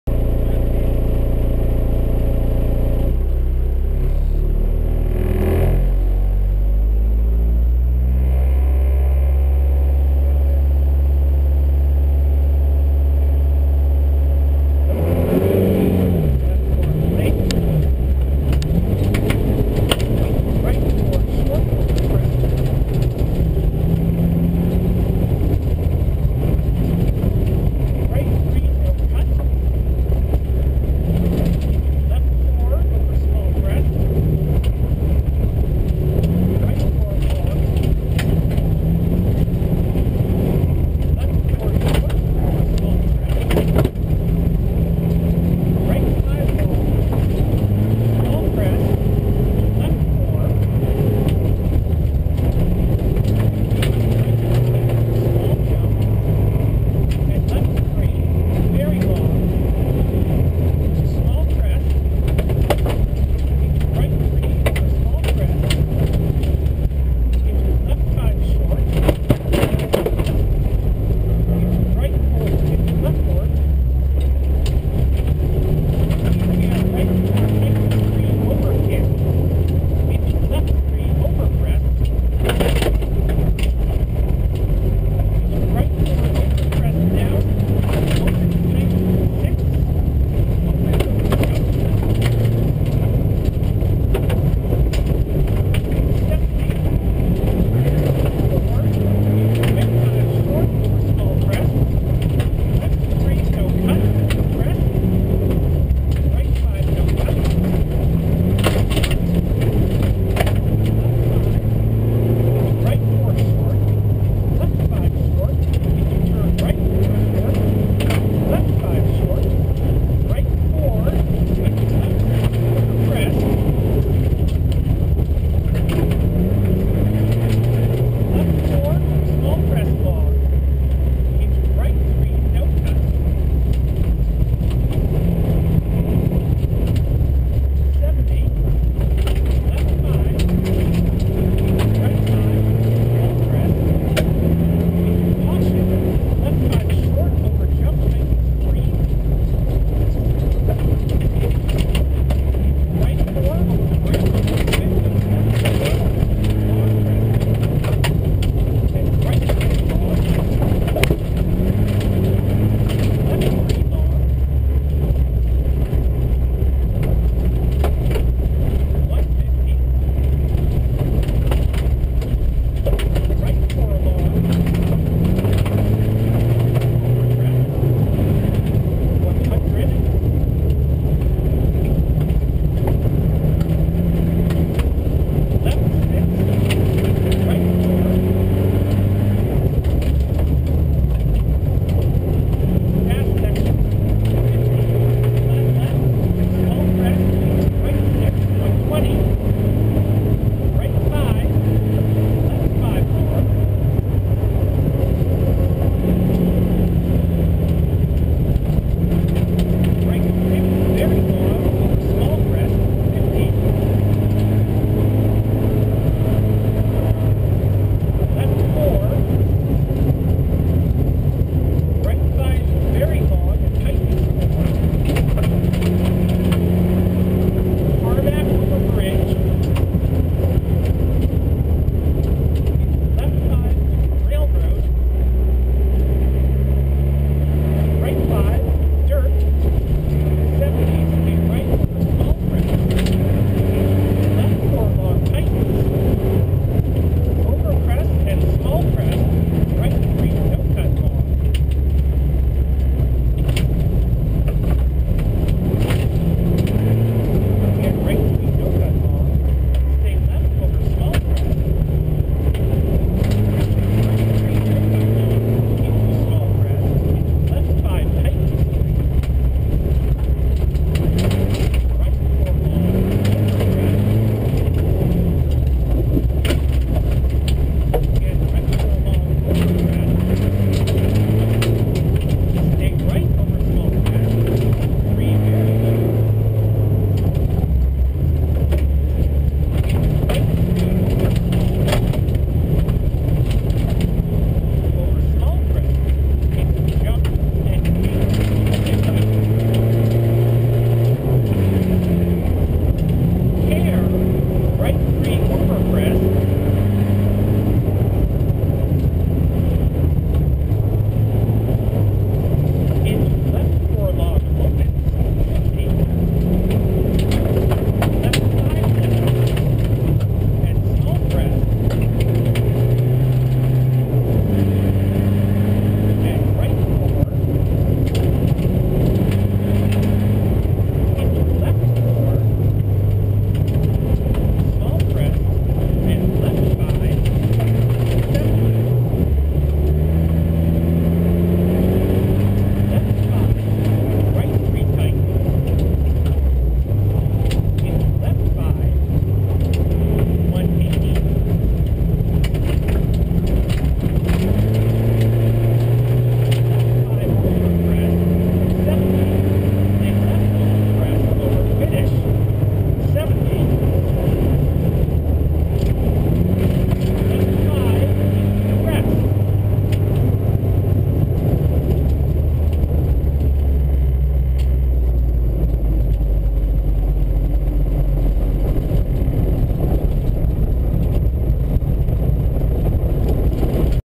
BRS 2013 SS16 - Greasecar Motorsports
In car from the Greasecar Motorsports Volkswagen Golf TDI on the second pass of the Jayville Out stage of the Black River Stages rally.